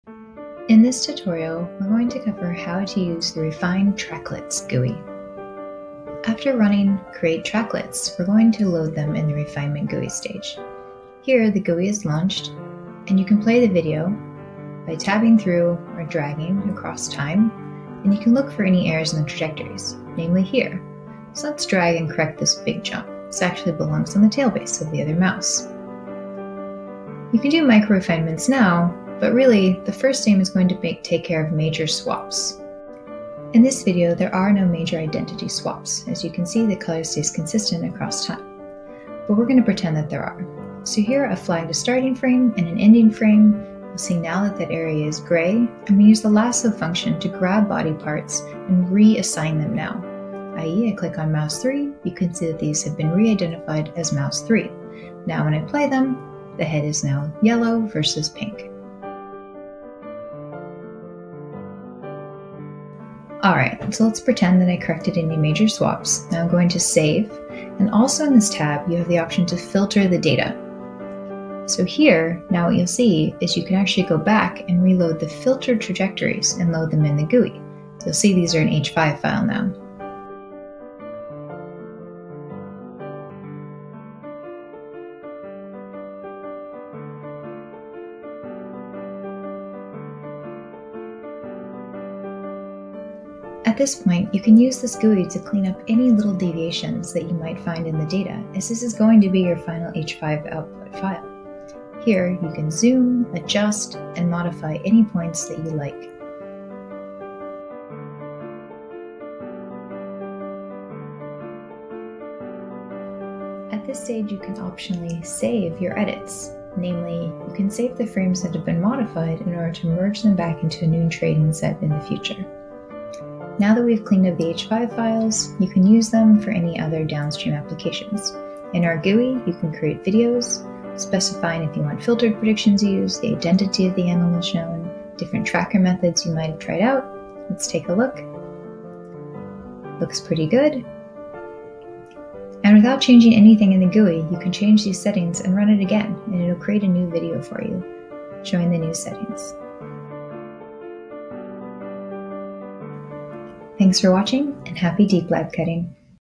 DeepLabCut 2.2: how to use the refine tracklets GUI!
This video describes the workflow after you have analyzed a new video in DLC2.2. Please for more information.